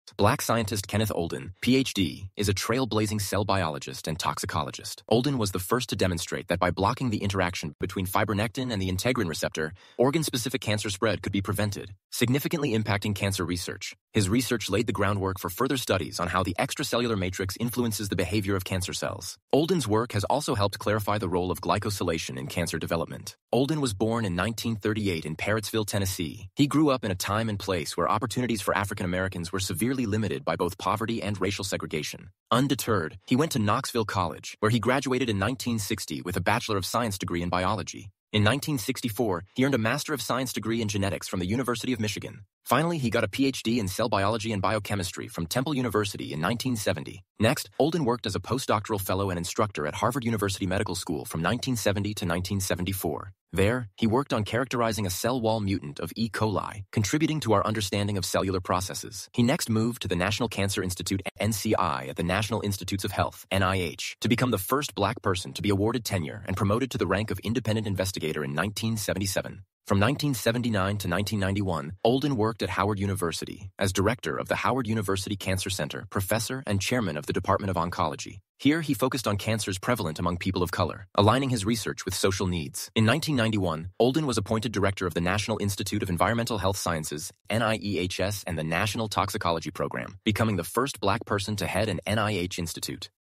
Transforming Public Health: Dr. Kenneth Olden's Contributions to Environmental and Cancer Research
Black Inventors: Kenneth Olden, PhD Pioneering Cell Biologist & Oncologist

Black scientist, Kenneth Olden, PhD is a trailblazing cell biologist and toxicologist.

Olden was the first to demonstrate that by blocking the interaction between fibronectin and the integrin receptor, organ-specific cancer spread could be prevented, significantly impacting cancer research.

His research laid the groundwork for further studies on how the extracellular matrix influences the behavior of cancer cells.

Olden’s work has also helped clarify the role of glycosylation in cancer development.

Olden was born in 1938 in Parrottsville, Tennessee.

He grew up in a time and place where opportunities for African Americans were severely limited by both poverty and racial segregation.

Undeterred, he went to Knoxville College, where he graduated in 1960 with a Bachelor of Science degree in biology.

In 1964, he earned a Master of Science degree in genetics from the University of Michigan. Finally, he got a PhD in cell biology and biochemistry from Temple University in 1970.

Next, Olden worked as a postdoctoral fellow and instructor at Harvard University Medical School from 1970 to 1974. There, he worked on characterizing a cell-wall mutant of E.coli, contributing to our understanding of cellular processes.

He next moved to the National Cancer Institute (NCI) at the National Institutes of Health (NIH), to become the first Black person to be awarded tenure and promoted to the rank of independent investigator in 1977.

From 1979 to 1991, Olden worked at Howard University, as director of the Howard University Cancer Center, professor, and chairman of the Department of Oncology. Here, he focused on cancers prevalent among people of color, aligning his research with social needs.

In 1991, Olden was appointed director of the National Institute of Environmental Health Sciences (NIEHS) and the National Toxicology Program (NTP), becoming the first Black person to head an NIH institute.